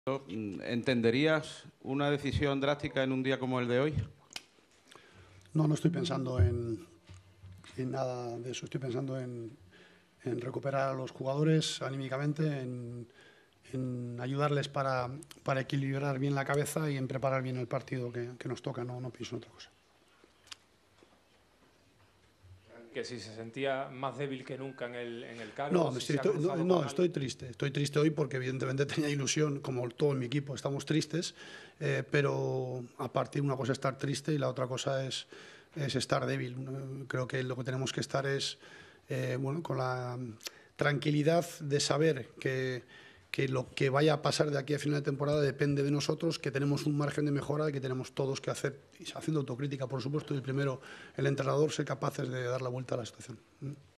LOPETEGUI HABLA con su FUTURO en el AIRE: JULEN, su CONTRATO y MONCHI | AS
Julen Lopetegui está viviendo el peor momento como entrenador del Sevilla. El técnico vasco habló así sobre su futuro tras dos preguntas sobre su contrato.